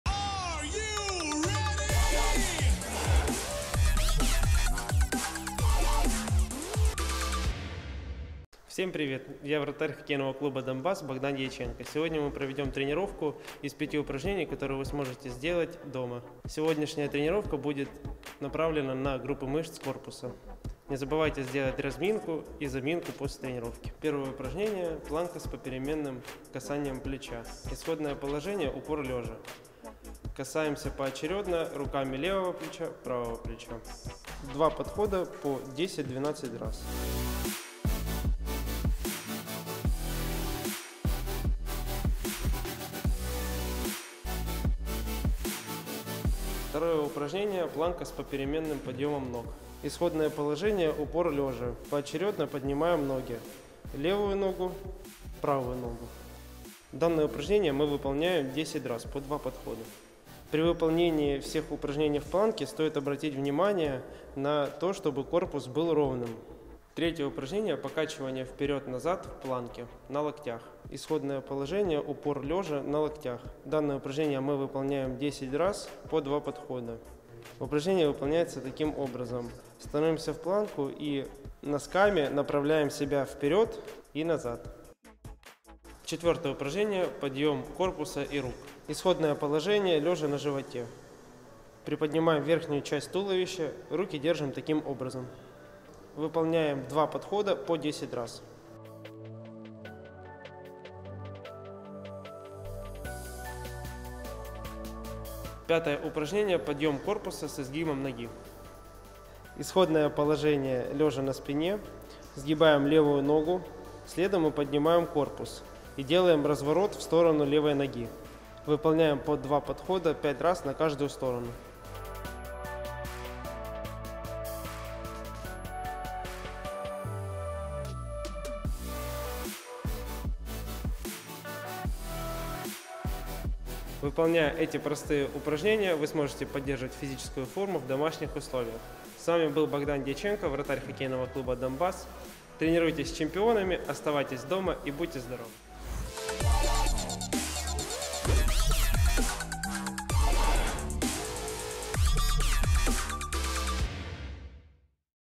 Уроки физкультуры у тебя дома | Богдан Дьяченко
Хоккейный клуб «Донбасс» и телеканал XSPORT продолжают совместный проект «Уроки физкультуры у тебя дома» - действующие чемпионы Украины показывают школьникам, как поддерживать форму в условиях карантина, а также межсезонья.

Очередное занятие проведет голкипер основной команды «Донбасса» Богдан Дьяченко.